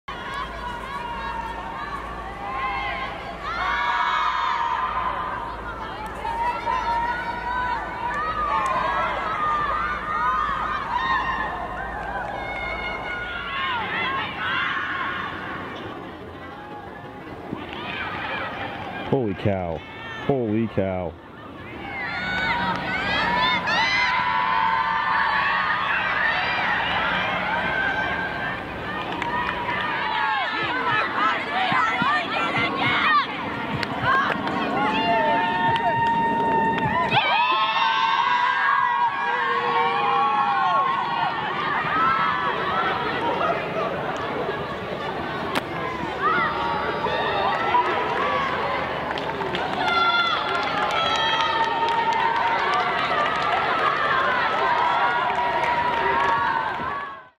Anne Worth v UCSD 2019 Northwest Challenge
Anne Worth showed off her speed for UNC's offense during their showcase game against UCSD during 2019 Northwest Challenge.